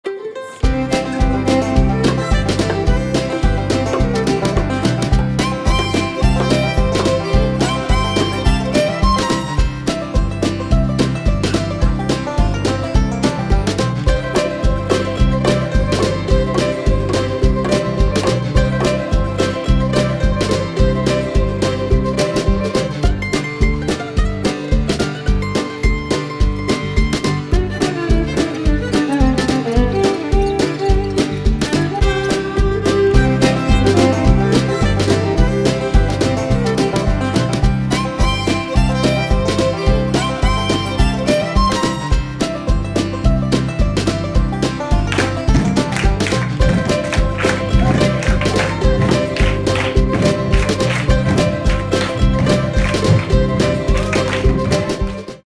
Snuffbox! Creative Lab's Storytelling Crash-course-experiment!
Creative Lab 2016; foundations of storytelling. Members were also challenged to think of different spaces, spacing and modes of audience address.